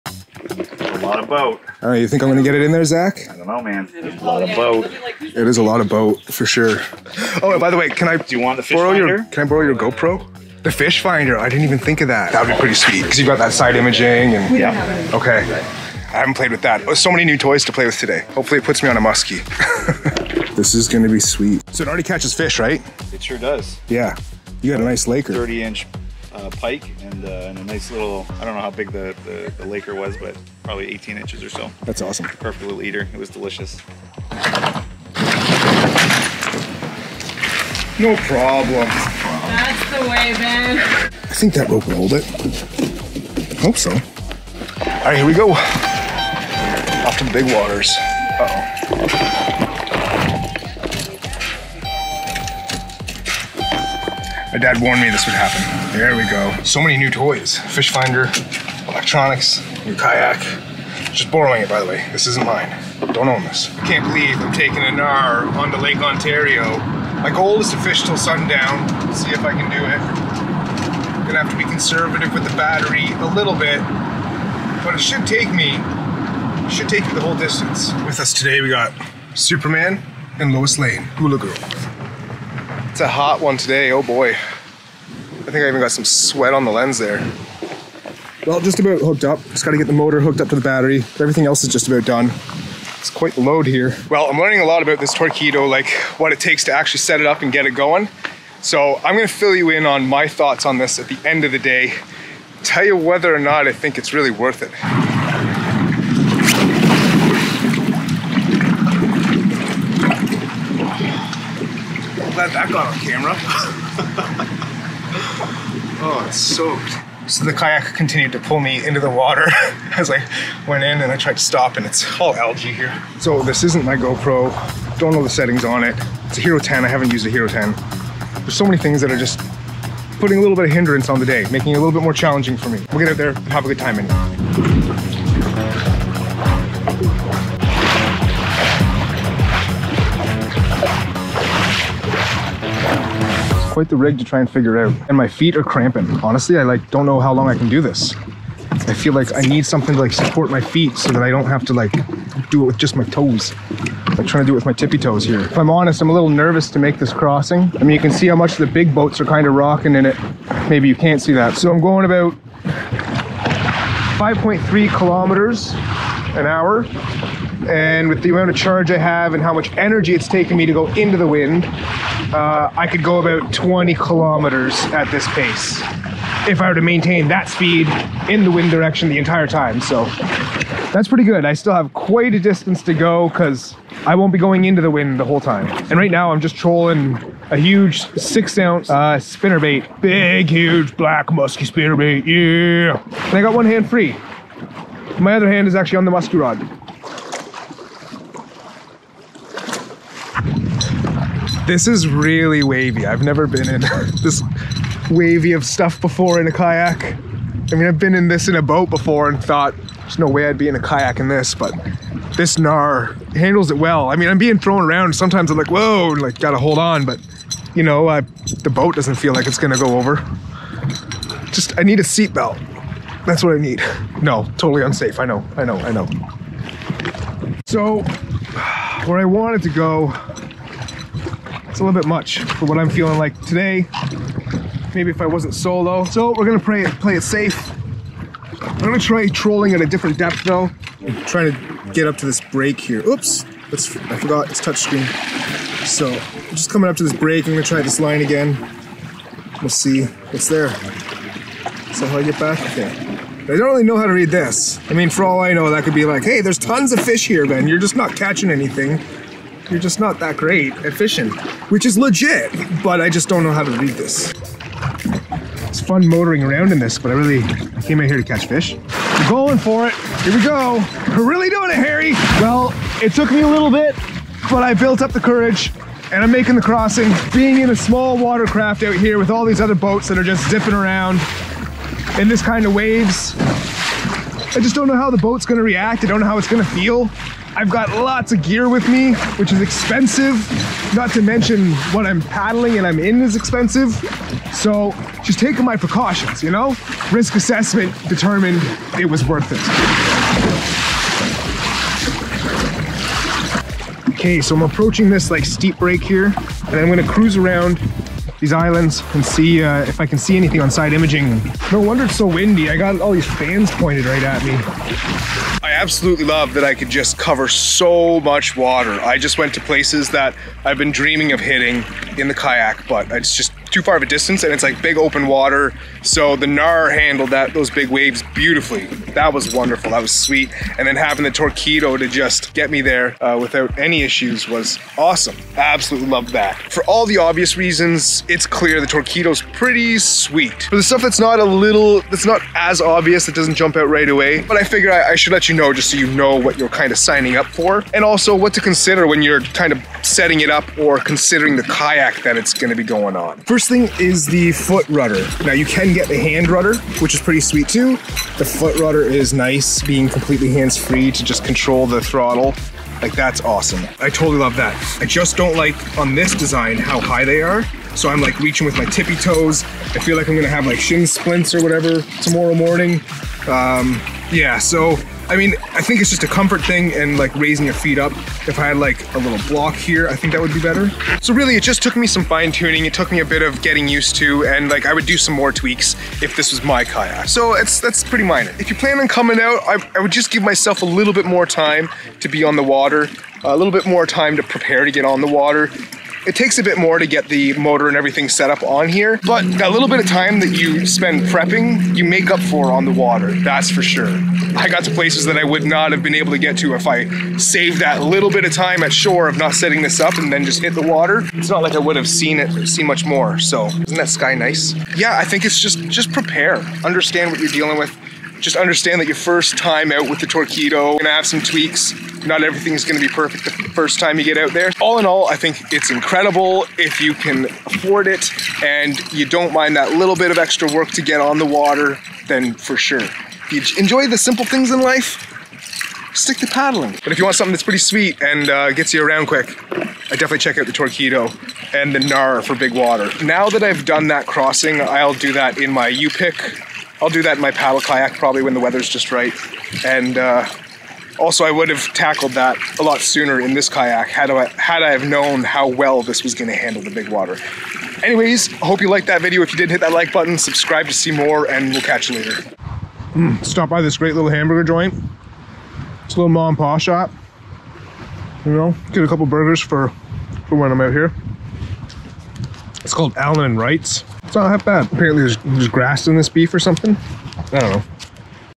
Jackson KNARR with a Torqeedo - First-day Thoughts
Ben borrows Zacks Jackson Knarr with a Torqeedo 1103 rigged up to chase some musky on Lake Ontario.

Check out these links for some of the gear we use and companies we support.

🐠 Soft Plastic Fishing Baits made in Ontario!